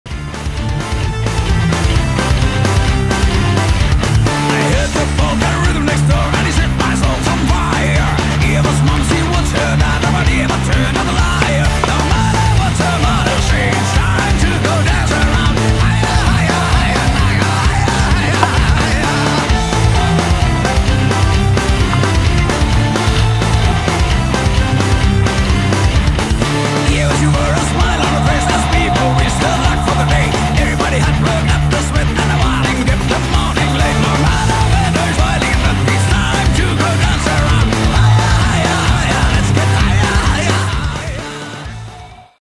Бодрый весенний сноупарк Уктуса
Раскатываем весенний парк Уктуса. Апрель это то время когда в парке народу больше чем на учебке)))